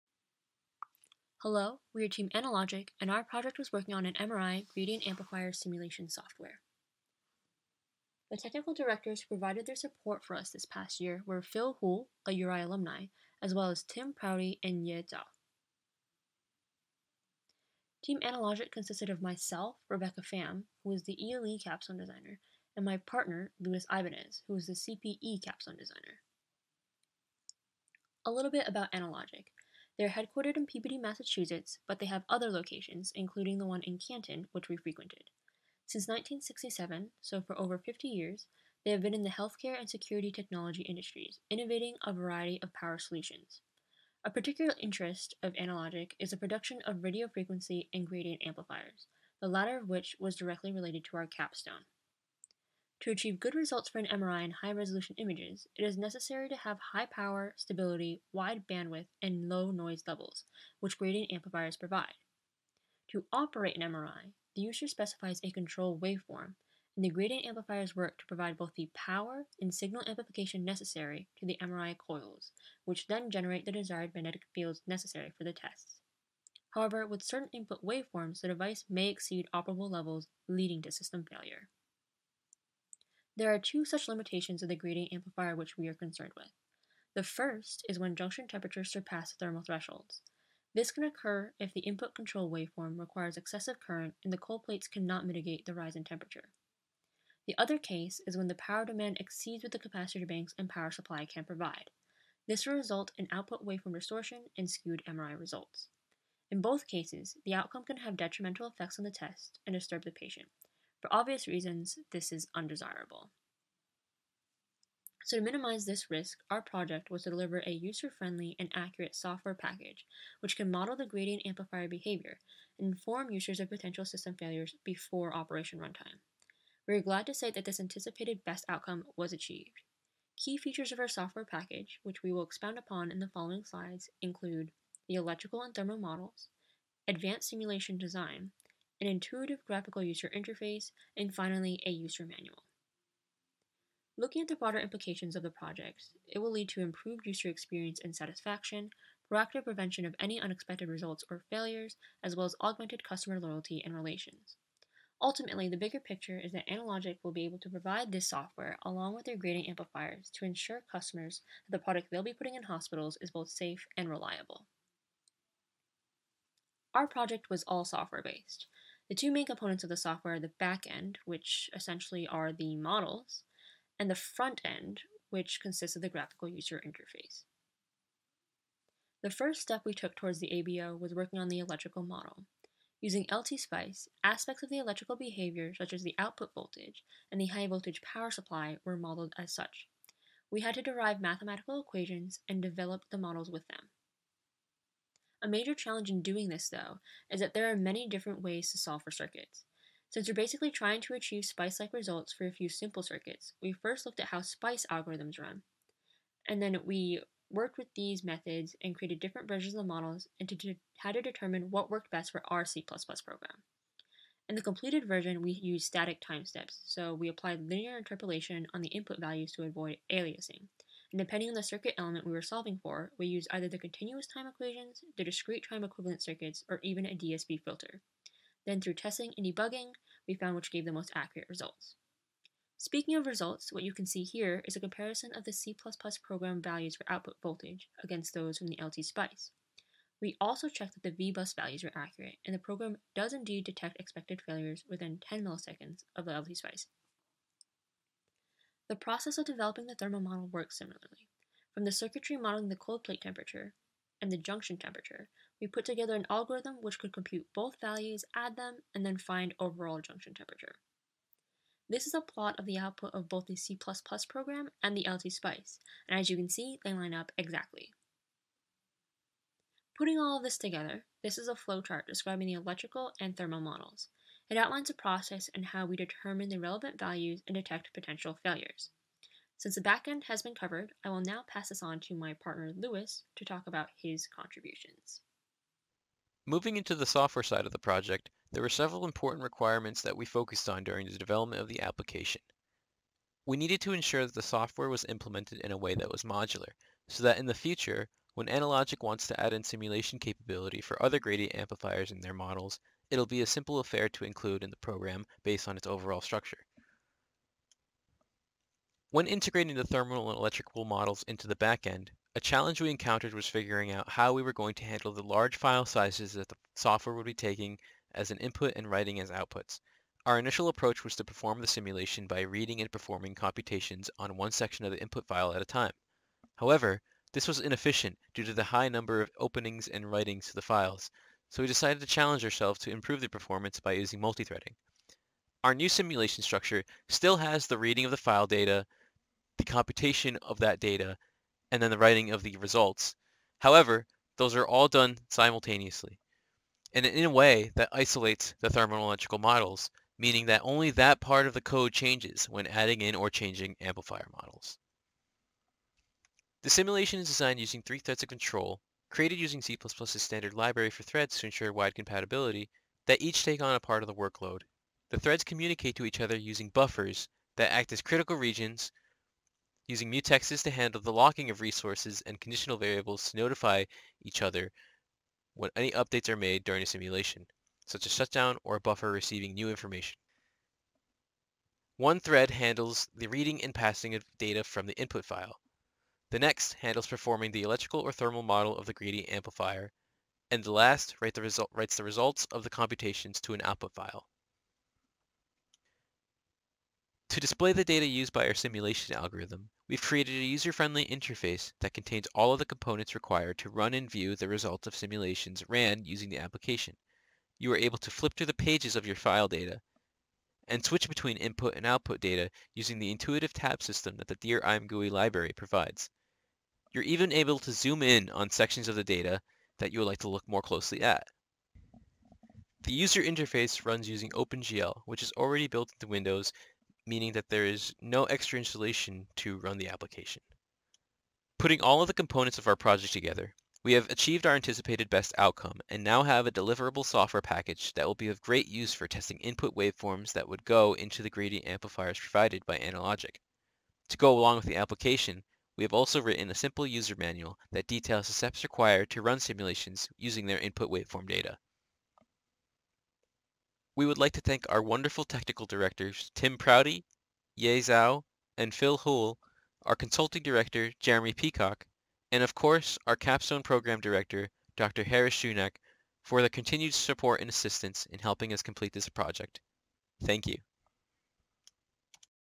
Thermal and Electrical Model for MRI Gradient Amps - 2020 ELECOMP Capstone Summit - Analogic
Senior Computer and Electrical Engineering students from the University of Rhode Island present their Capstone Design projects. Due to COVID-19, the ELECOMP Capstone Summit was unable to be held in-person. Each of the 16 teams has instead created a virtual presentation to showcase their accomplishments in engineering design.